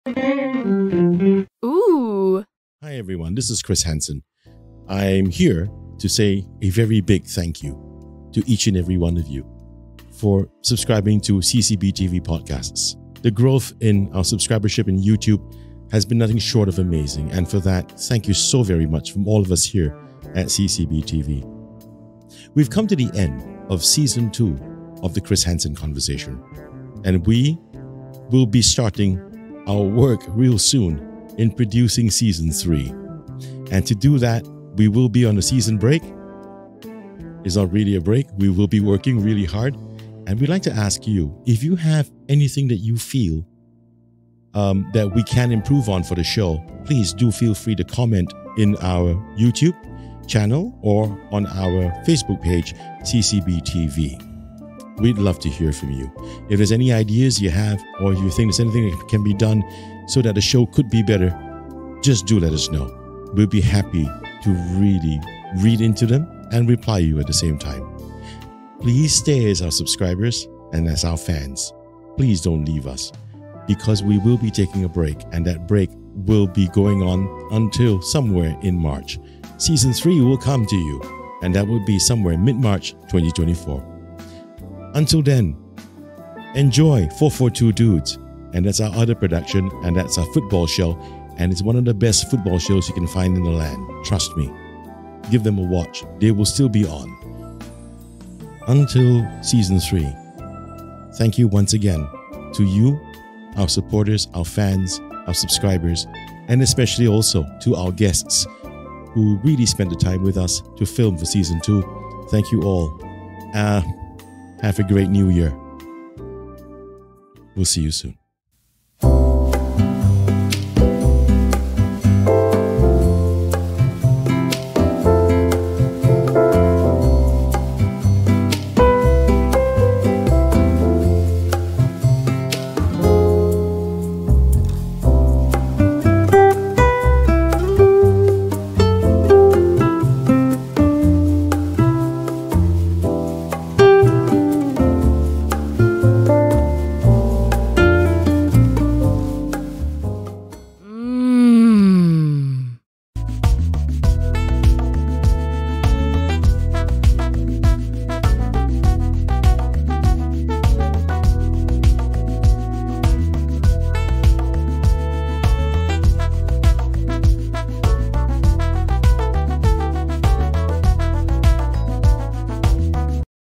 Thank You Everyone for A Wonderful Season! | The Chris Henson Conversation - End of Season 2
Season 2 of The Chris Henson Conversation has ended! We are now planning and producing Season 3 during the Season Break! Rest assured, The Chris Henson Conversation will return somewhere in March 2024!

It would be remiss of us to not thank all of our Supporters, Fans and Subscribers from all over the world for helping CCBTVPodcasts achieve not one but several milestones in our subscribership in just two months!

We are still considerably a new show and CCBTV is still learning. Do give us comments and help tell us how and where you think we can be better however, we will not deviate from producing good shows that are candid, authentic and entertaining therefore to give our viewers the fullest experience, we intend to keep our episodes in the long-form podcast format and continue to provide excerpts, shorts, teasers and audio versions of our shows!

Thank you once again from Chris Henson, 4-4-2Dudes, Khai Rules and everyone from CCBTV! Have A Happy and Healthy 2024!